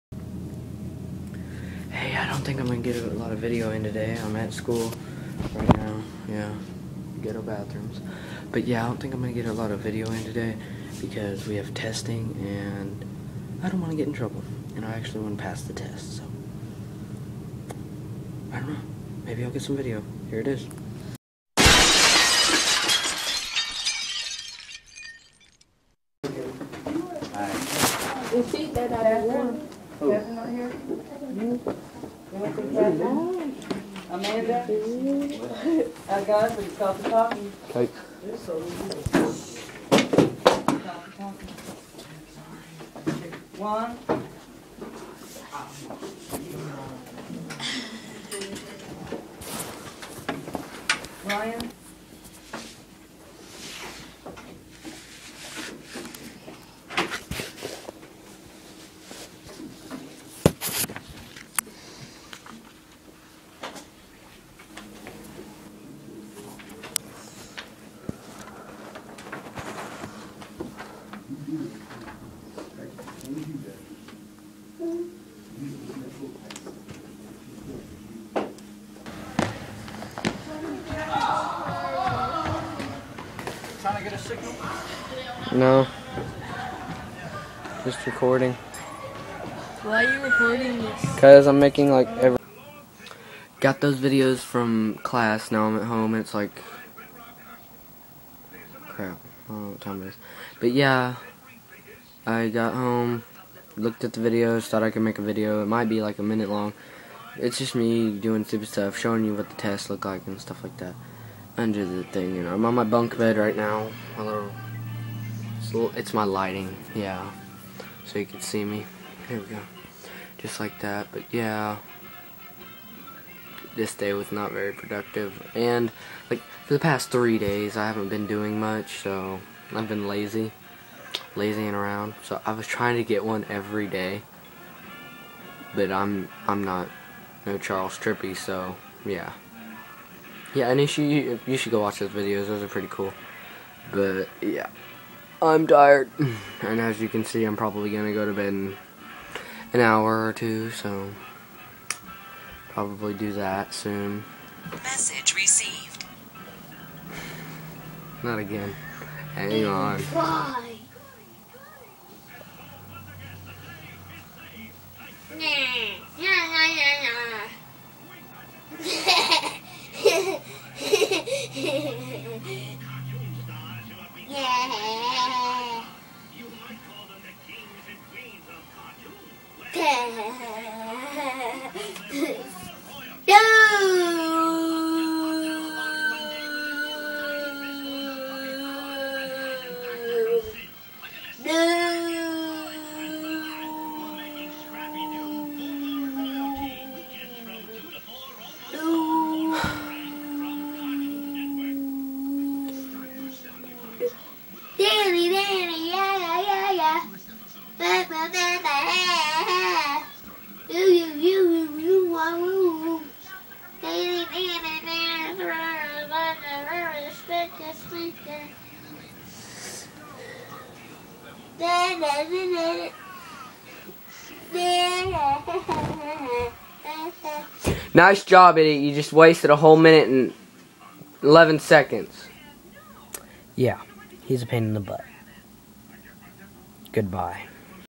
In class test
Me in class with my ipodtouch being wierd try hard at a test